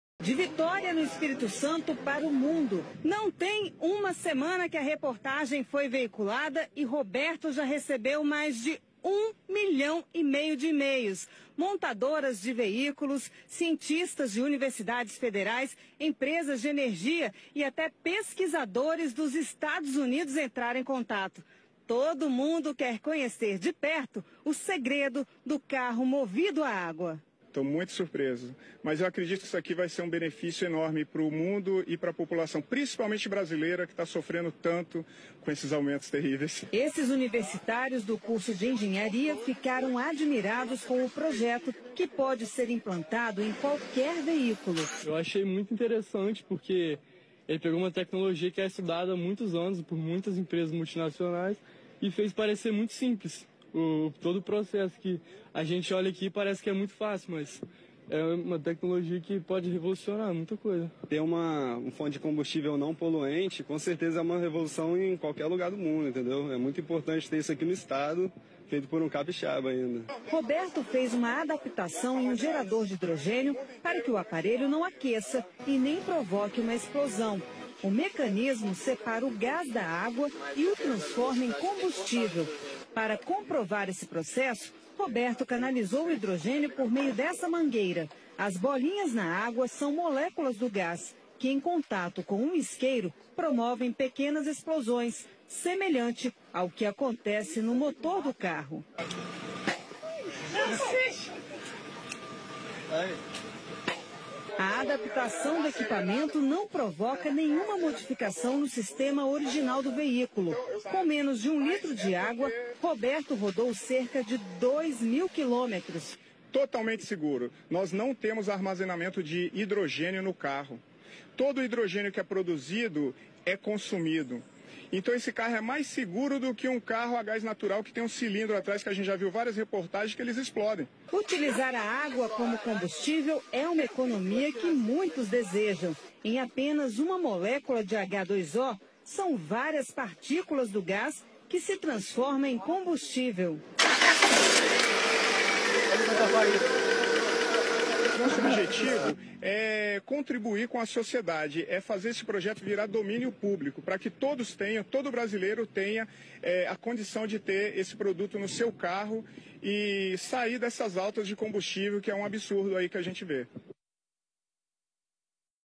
Carro movido a Água já é possível! Veja a descrição do vídeo
Carro movido a água já é possível site especializado tem o kit chega de combustível com valor absurdo! vale a pena pelo menos analizar, motor movido a Água é um sonho possivel